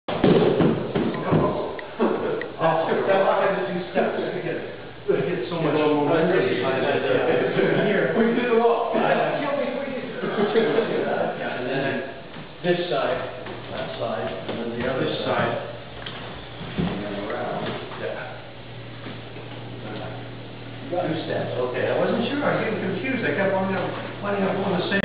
Short Clip from Bagua Class 2008
An old clip I found on my computer, working on different jins in class and demonstrations...